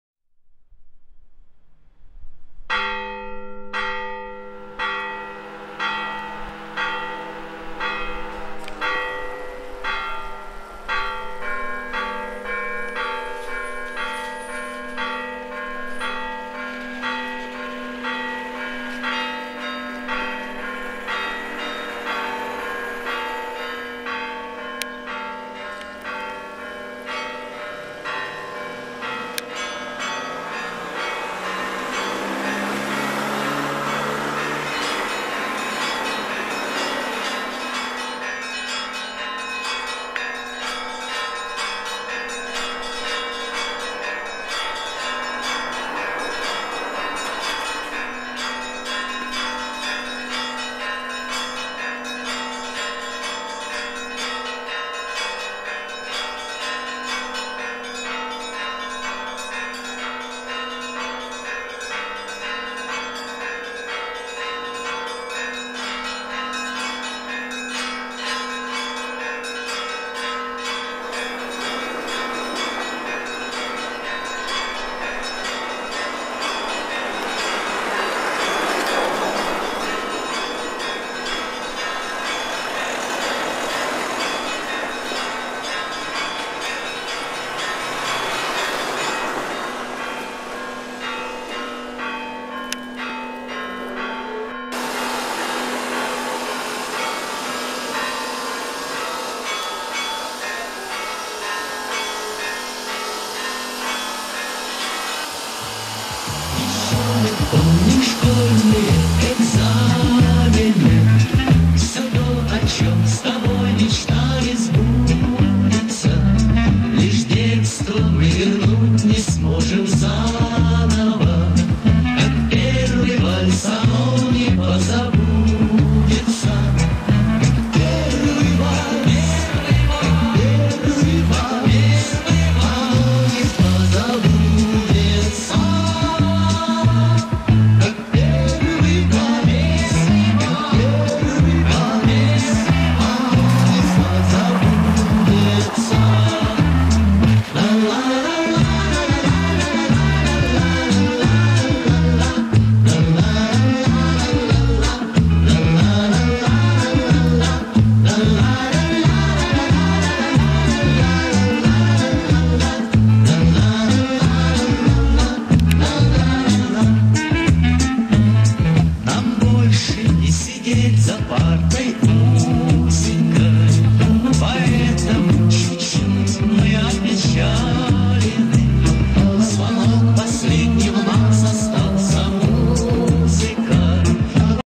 ПРИВЕТ ИЗ НОВОЙ КАХОВКИ 5 Л В М 2014
Украина,Новая Каховка.Город на Днепре.Храм Андрея Первозванного